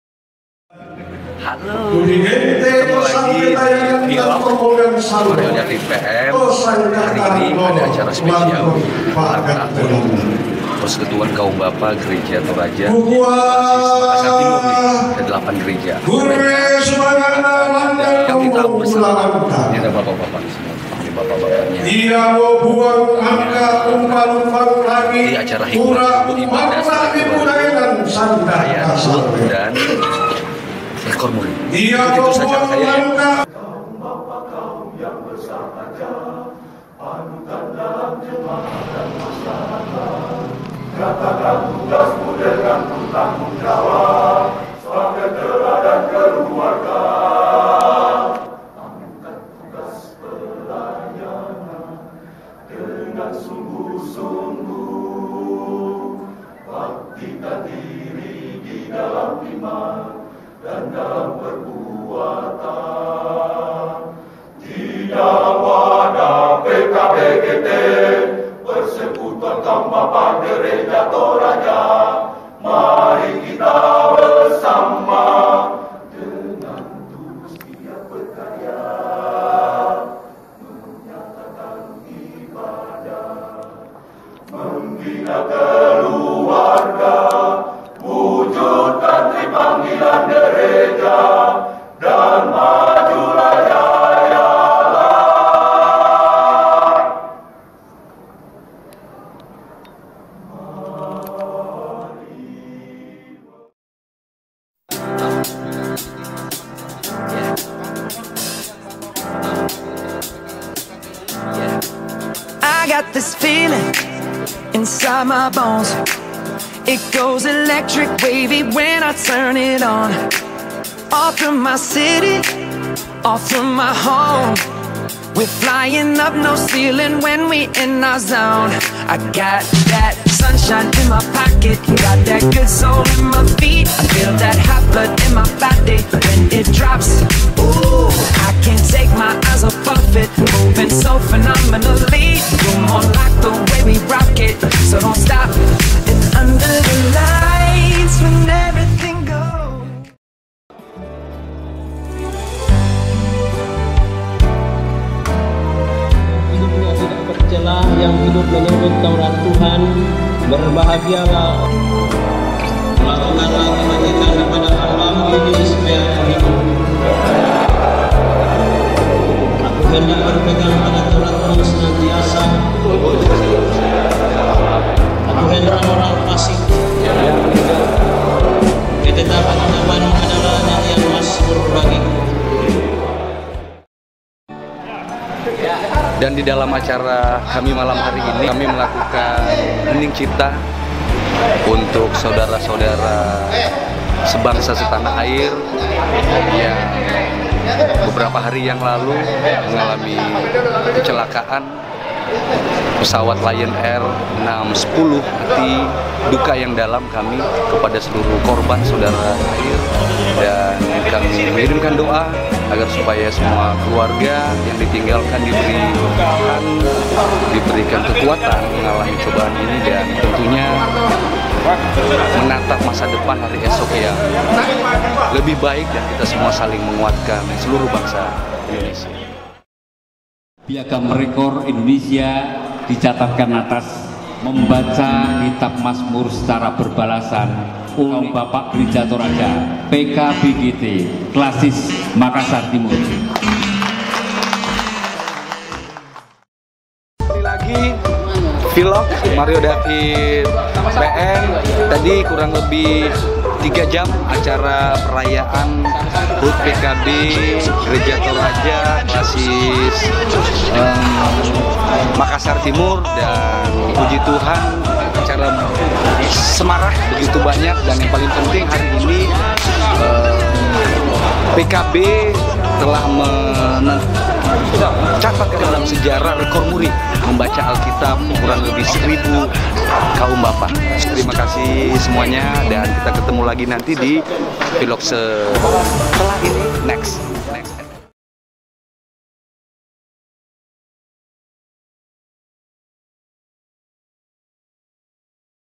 Mario David Vlog
Menghadiri Ibadah PKB GT Makassar Timur @Balla Tamalanrea

Selamat yah... PKB GT Dapat Rekor MURI ^_^